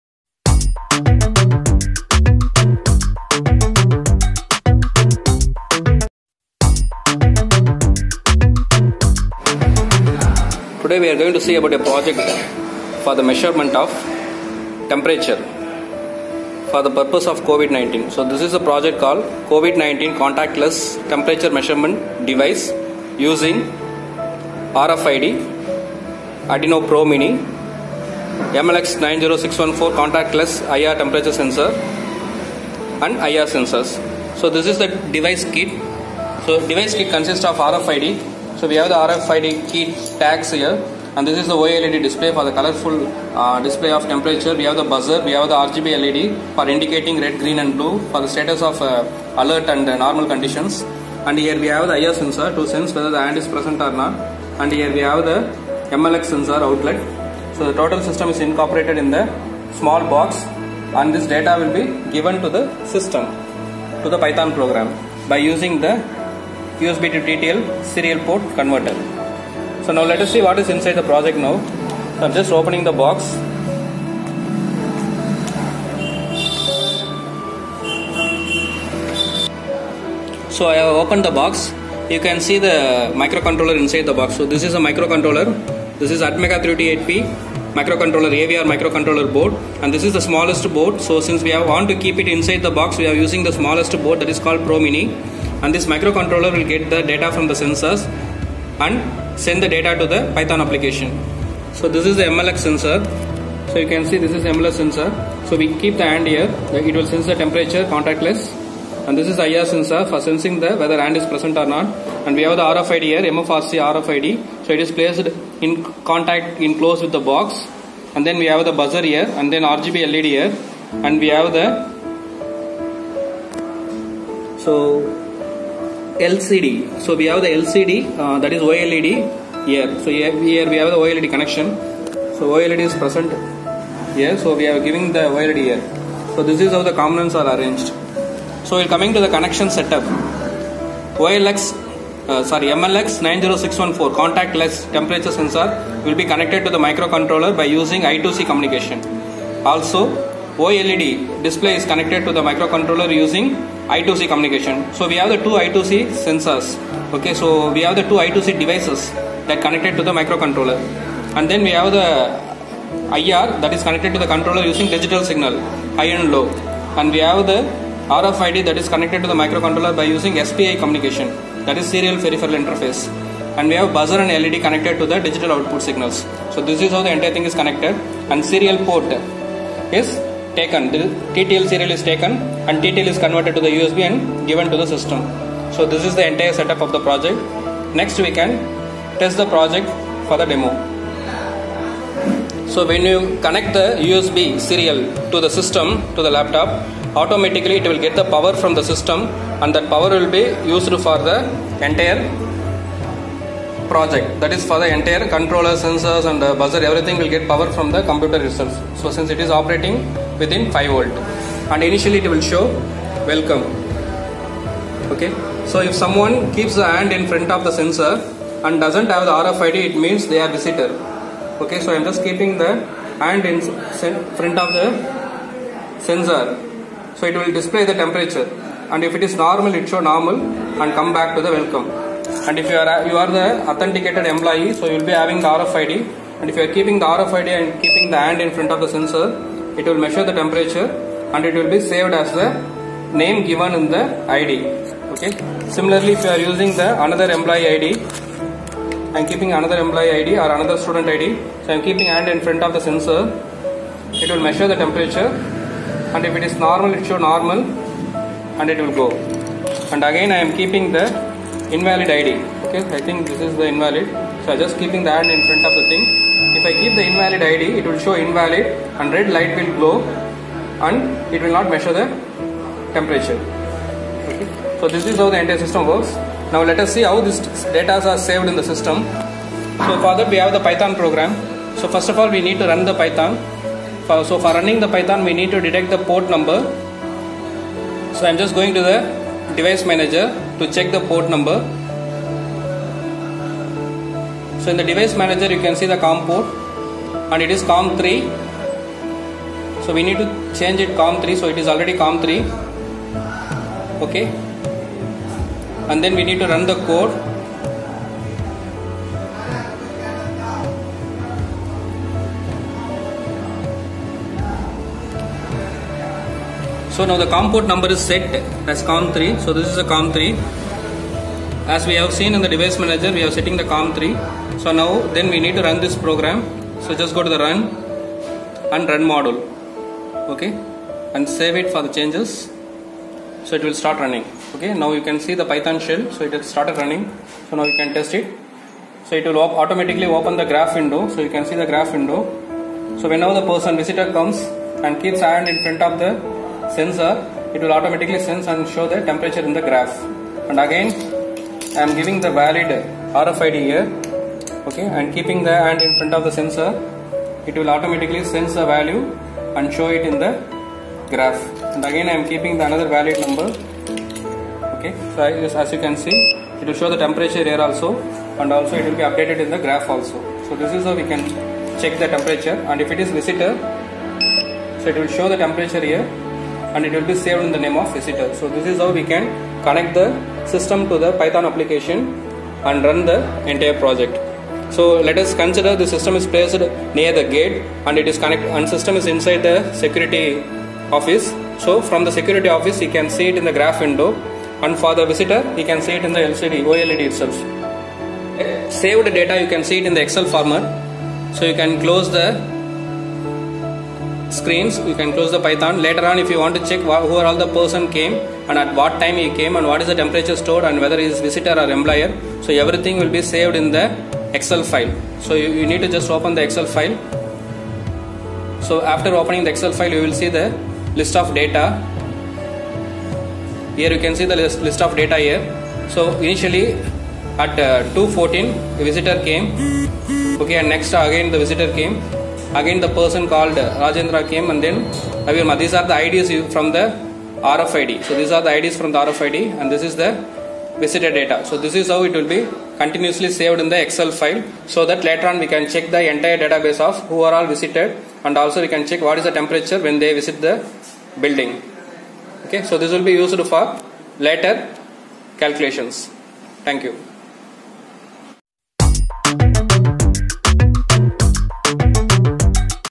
Infrared Body Temperature Device - MLX90614, Arduino, RFID & OLED with Python Graph & Excel Data
Infrared Body Temperature Device - MLX90614 IR sensor Arduino Pro Mini, MFRC522 RFID, LED lights & OLED display with Python Graph & Excel Data